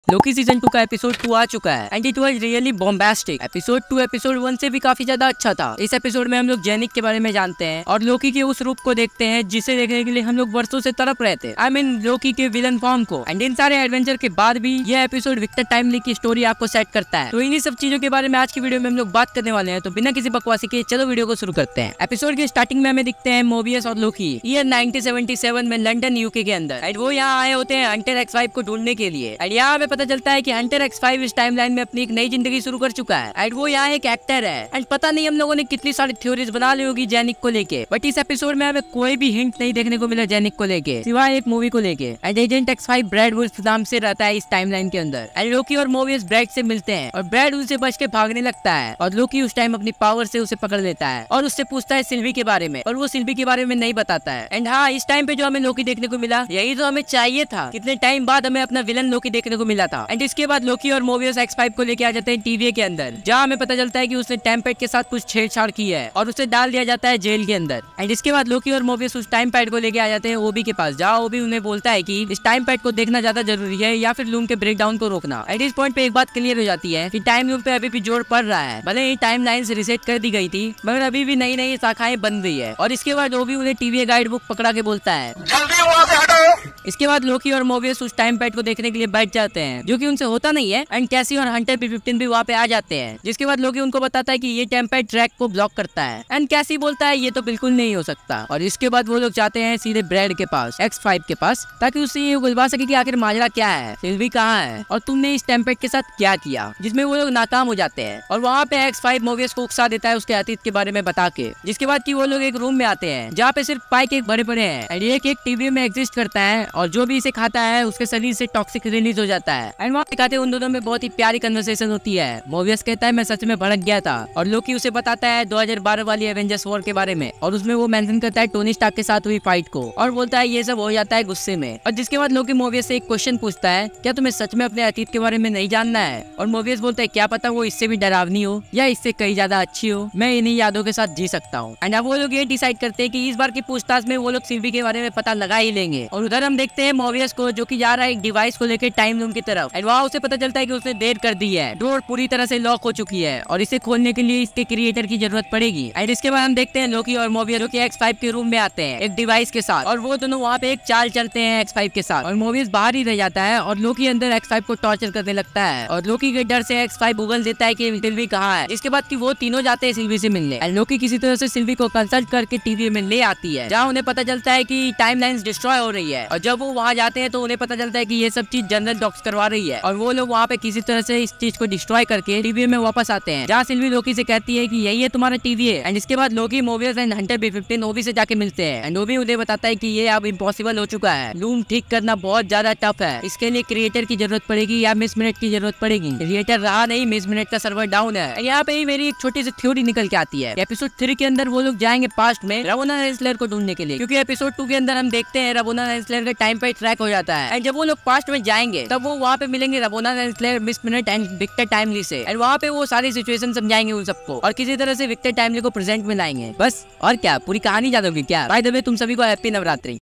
Ye Unexpected Tha 😱!Loki Season2 Episode 2 Explained In Hindi || SuperFreaky
This video is about loki season 2 episode 1 full explanation in hindi.I hope you all like it.